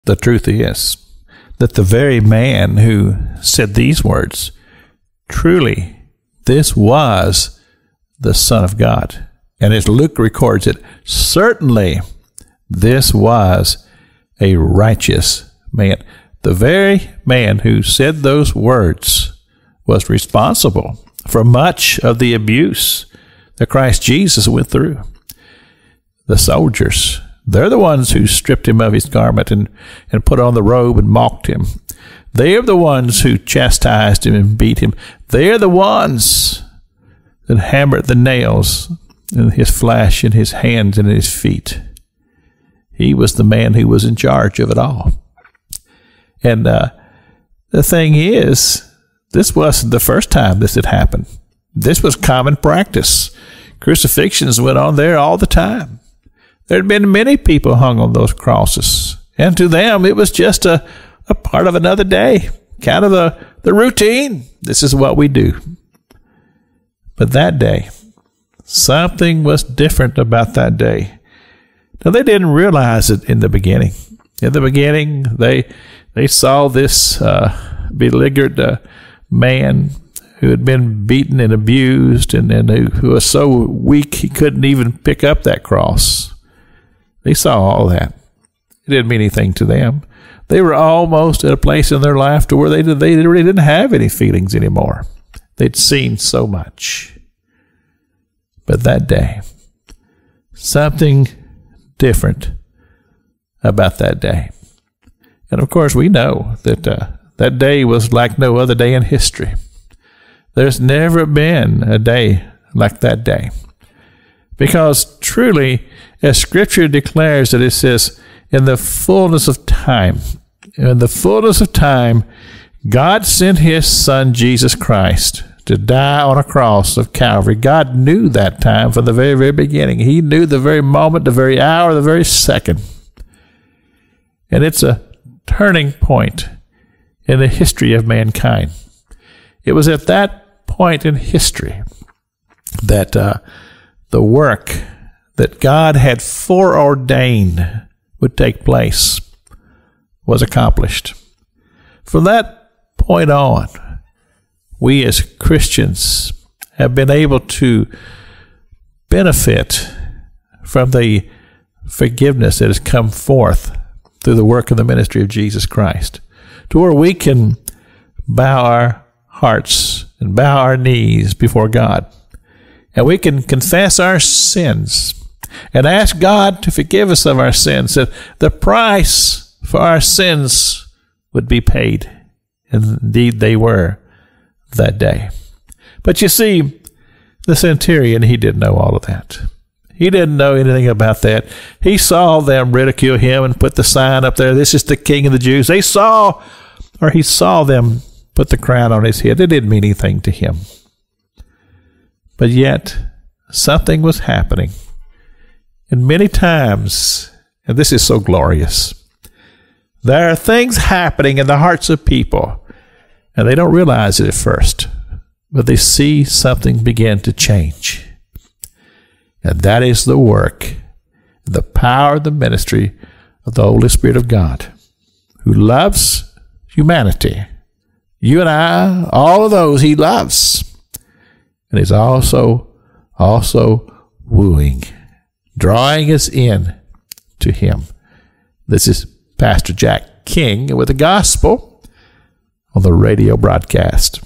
The Basic Fundamentals of Spiritual Life - Part 647
Let's continue talking about the Roman centurion who watched Jesus die on the cross.

By the way, if you haven't bought a copy of my new book yet, check it out